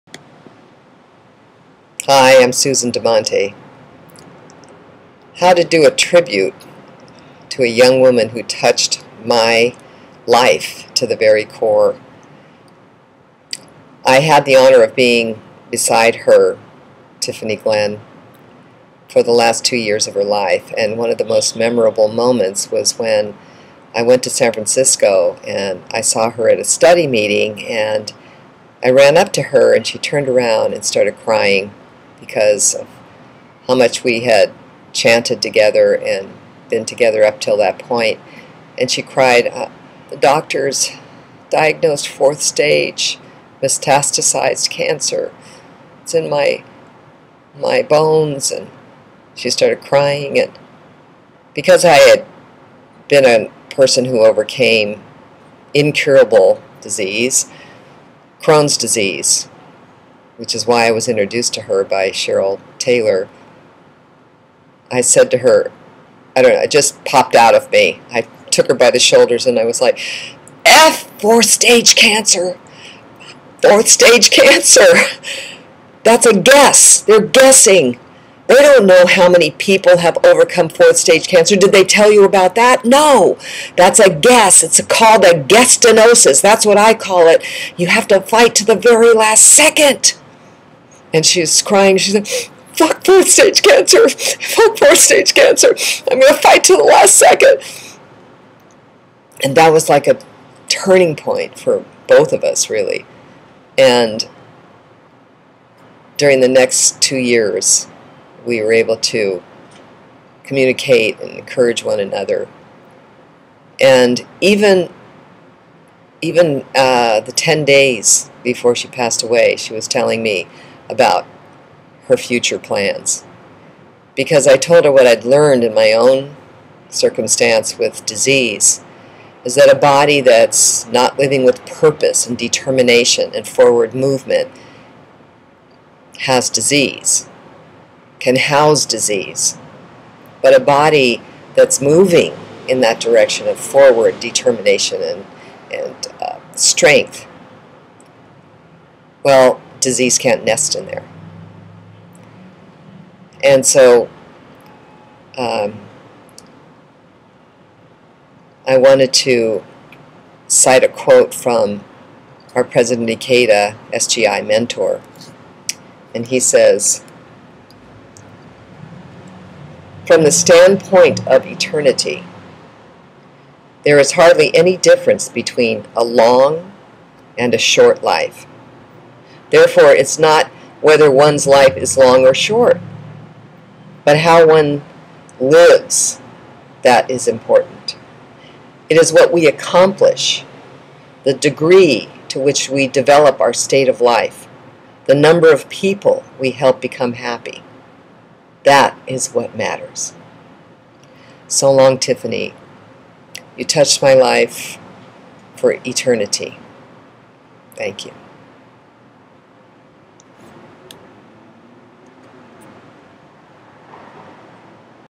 Tribute to Tiffany Glenn
Susan Damante's tribute to fighting alongside Tiffany Glenn.